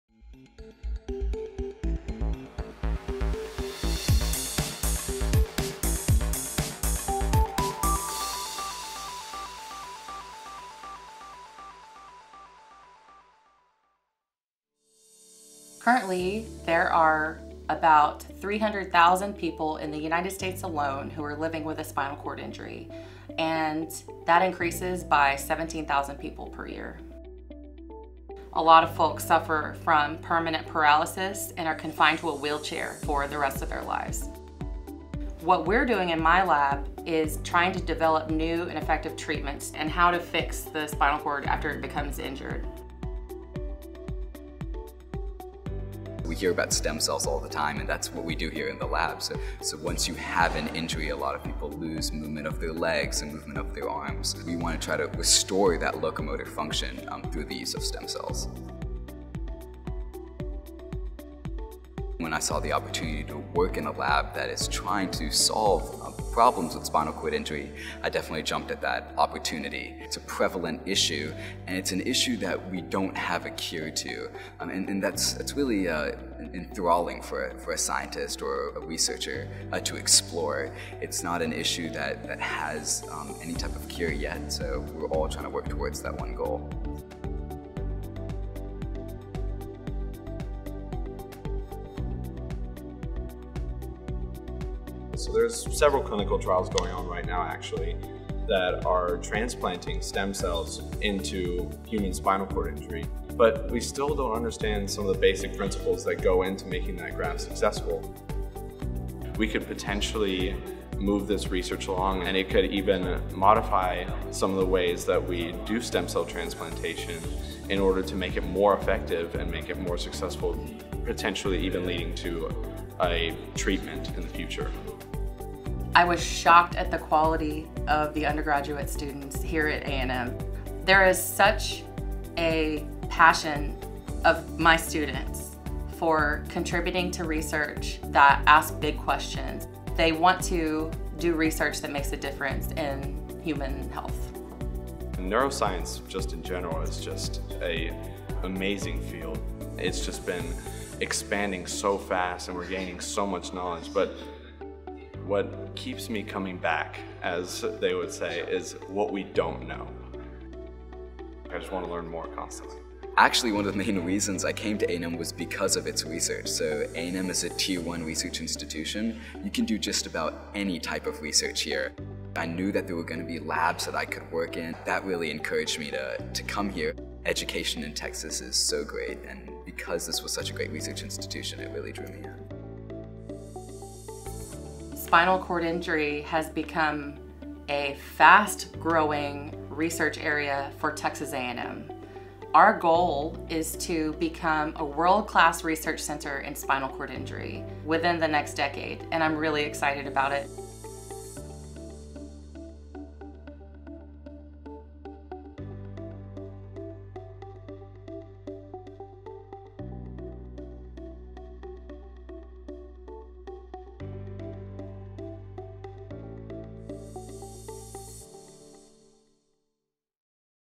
Labors of Lab - Sunjay Letchuman '22 & Michael Pitonak '22 (Episode 37)
In our latest Labors of Lab episode, Texas A&M students Sunjay Letchuman '22 and Michael Pitonak '22 discuss their roles in the research group of biologist Jennfier Dulin '05, which investigates ways of treating spinal cord injuries.